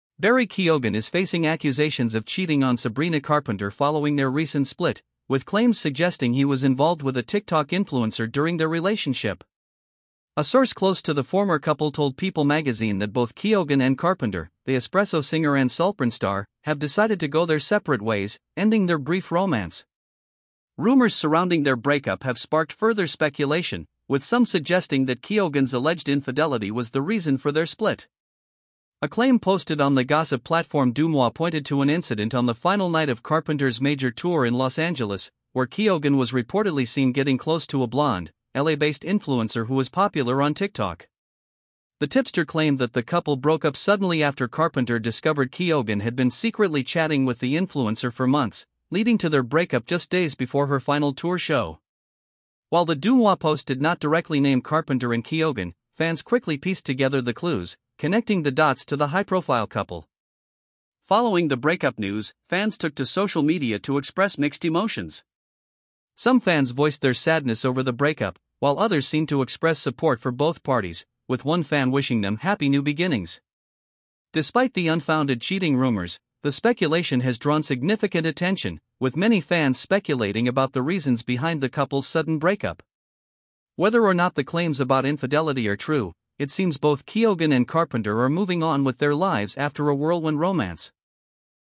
Sabrina Carpenter's fans slap Barry Keoghan with 'cheating' claims
In this video, we dive into the recent split between Sabrina Carpenter and Barry Keoghan, which has sparked rumors and controversy. Fans have accused Barry of cheating on Sabrina with a TikTok influencer, following a gossip claim on Deuxmoi. The speculation comes after the couple ended their relationship, which had been making headlines due to their whirlwind romance. We discuss the details behind the breakup, the cheating accusations, and the reactions from fans on social media.

Watch to find out more about what might have led to their sudden split and how both Sabrina and Barry are moving on.